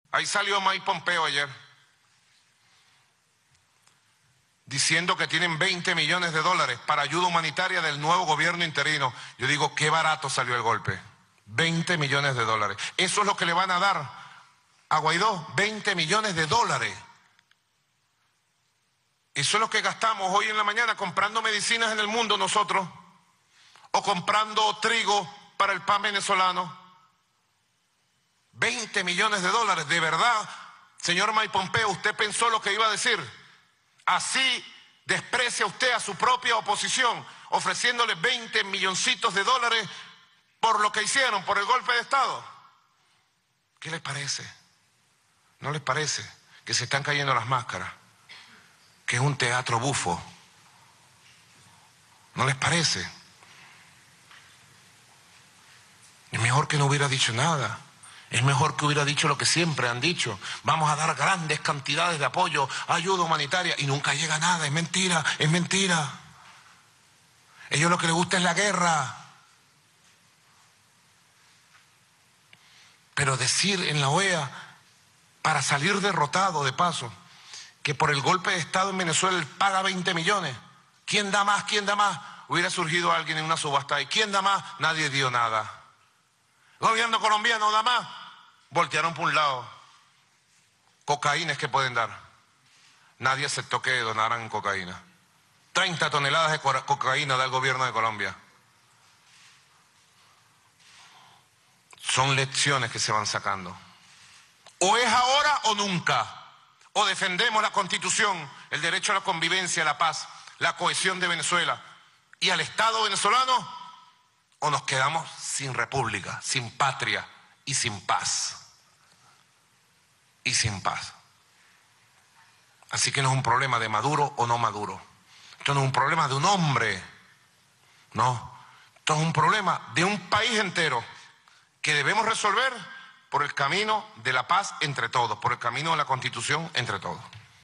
Maduro: ¿Estados Unidos ofrece $20 millones? Eso gastamos esta mañana en medicinas
El Presidente de Venezuela, Nicolás Maduro, critica en rueda de prensa este 25 de enero de 2019 a Mike Pompeo, secretario de Estado de Estados Unidos, tras ofrecer en la OEA, a nombre de su país apenas 20 millones de dólares como supuesta "ayuda humanitaria" para el país. Señala que ese dinero es, en realidad, lo que le está ofreciendo a Juan Guaidó para que dé el golpe de Estado.